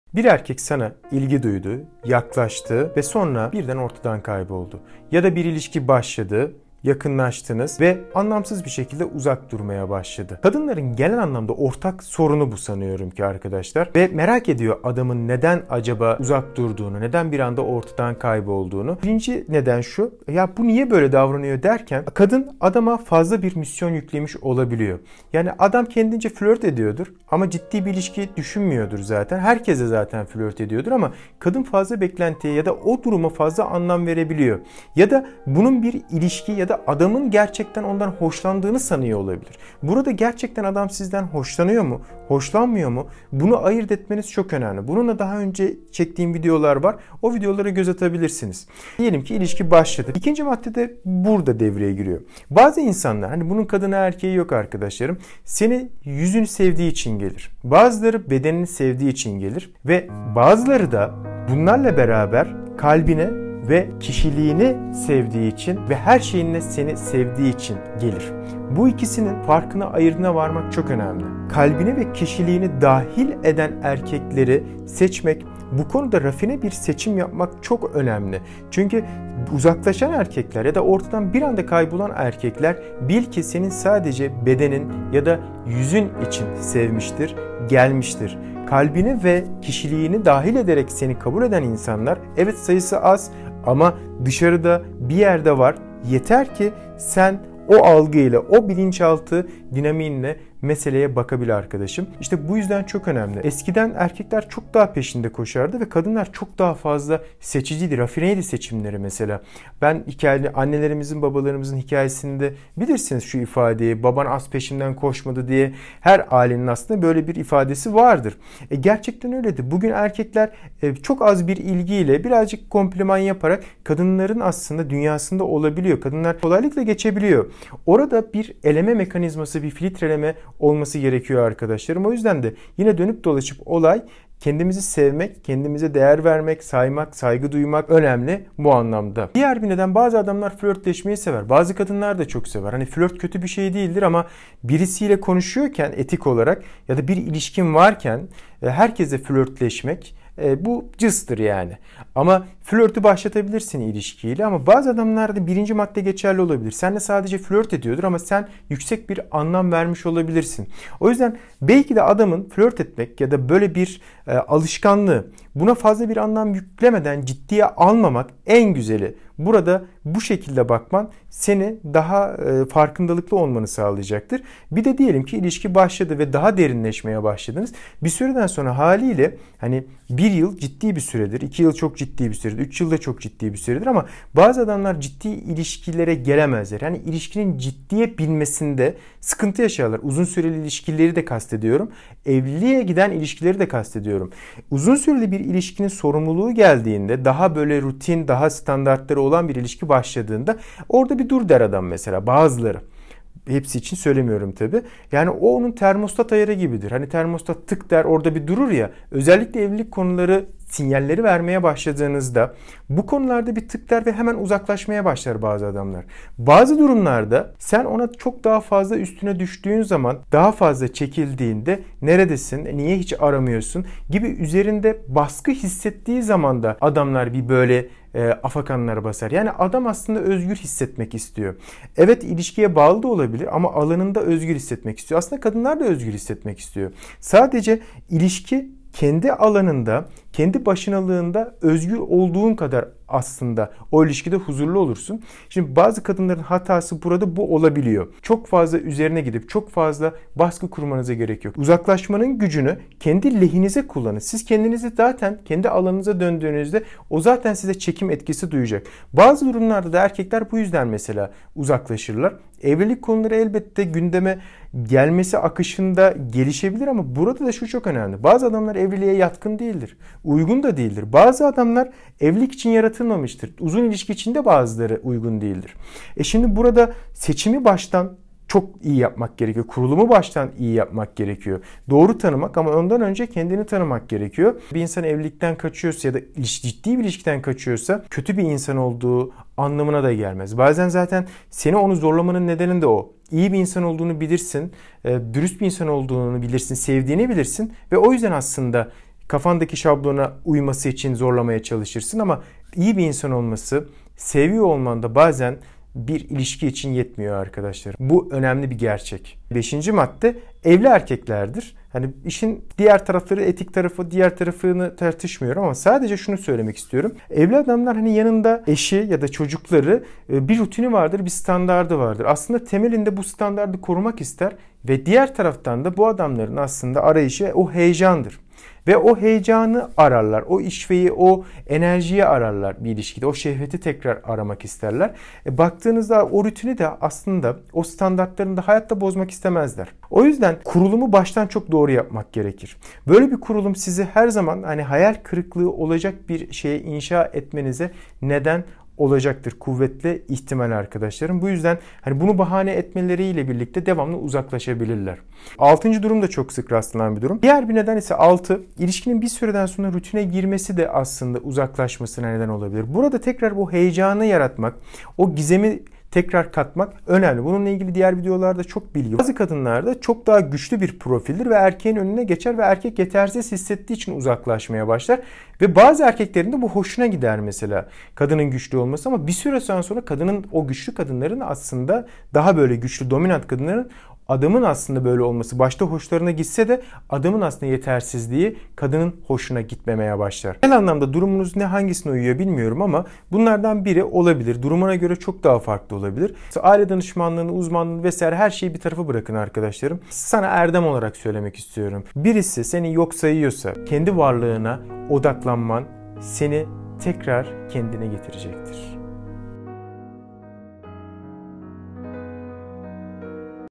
ERKEKLER NEDEN UZAKLAŞIR ? 6 NEDENİ ANLA | Yokluğu Seni Güçlendirsin !
Bir erkek neden uzaklaşır bugün ki ilişki videomuzun konusu erkeklerin dilini çözmek üzere olduğu gibi kadınlar hakkında da ipuçları veriyor. Bir erkek soğuyorsa ya da uzaklaşıyorsa bunun sebepleri her zaman kötü niyet değildir ama önemli olan senin seçimlerindir. Uzaklaşan erkekleri anlamak için güzel bir ilişkiler videosu oldu. İyi seyirler arkadaşlarım..

| Erdem Taşkınsu Kimdir ? Hangi Eğitimleri Almış- Ne Mezunudur ?

▶️ İnstagramdaki kişisel gelişim paylaşımlarımız için ;